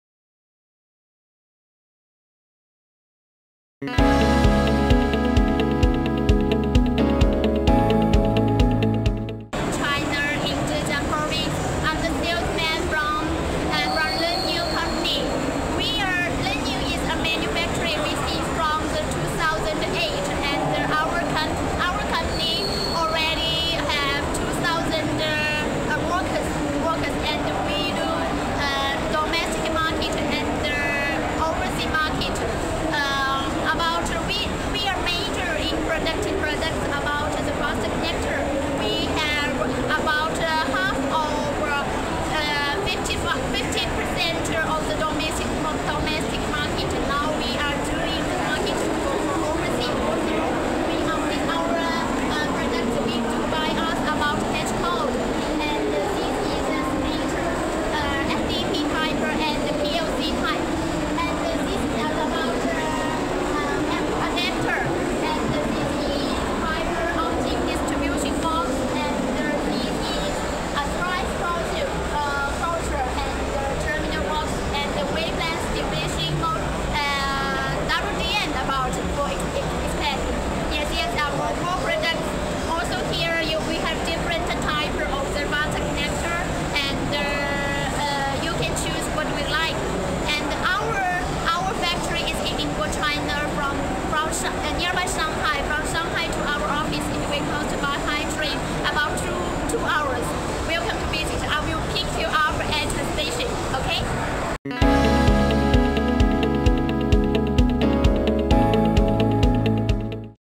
ICT COMM 2017 Vietnam: Maggie Weng from Lenew Electronic Technology
ICT COMM 2017 Vietnam: Maggie Weng from Lenew Electronic Technology talks about its product showcase at ICT COMM Vietnam